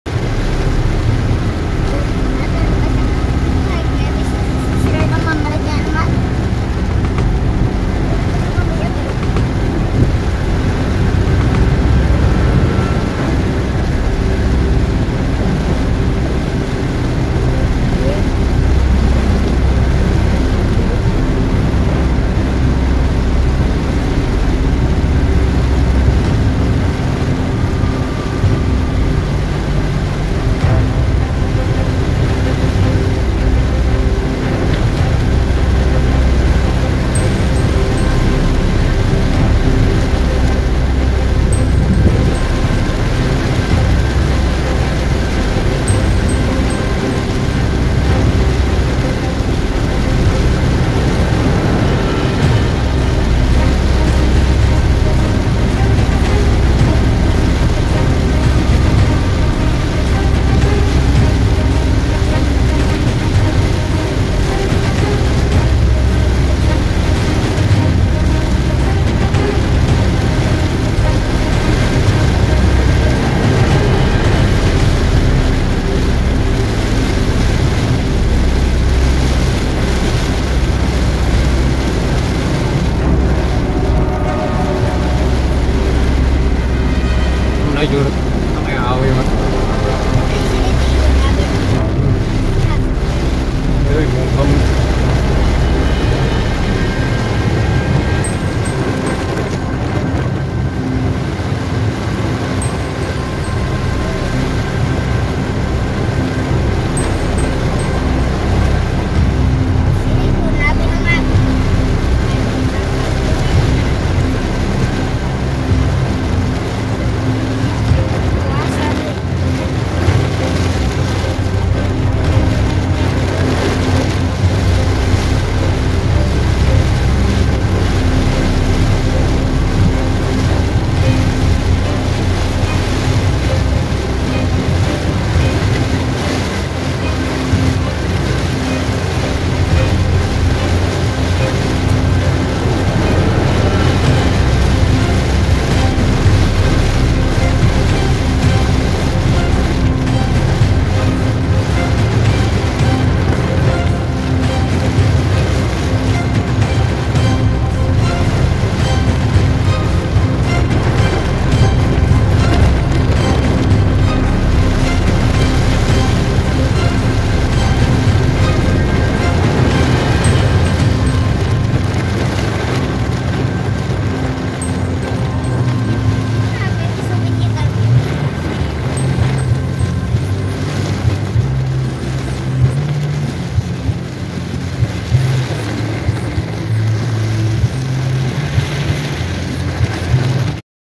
Waktu Terasa Mundur! Hujan Deras Seperti Mengucur dari Langit
Hujan paling deras dan besar di mobil.